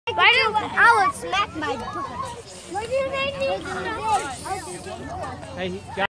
KENNEDY's 1st at BAT ! * 2018
John Kennedy Adams' 1st Baseball Hit Ever !  2018